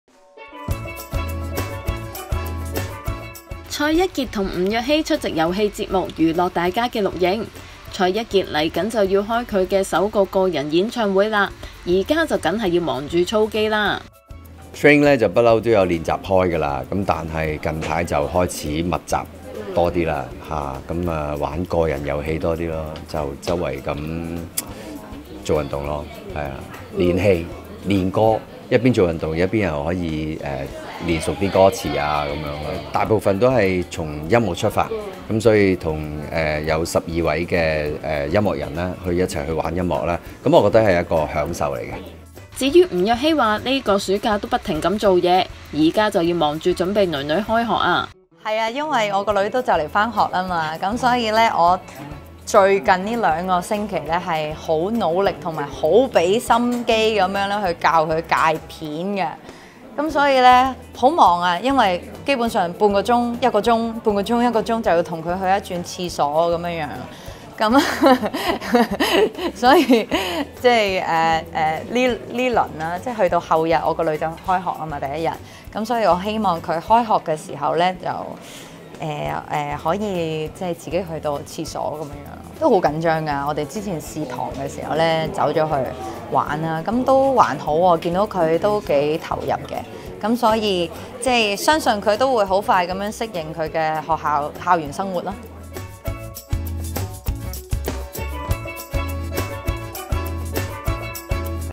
【娛樂】囡囡開學 吳若希 : 希望佢識去廁所 2019-08-14
吳若希出有席遊戲節目錄影，提到暑假忙緊為囡囡準備開學，佢話 : 「囡囡就快返學，最近好努力教佢戒尿片，每小時都要跟佢去廁所。到佢返學時，希望佢自己識得去廁所啦。」

bmgproductionmusic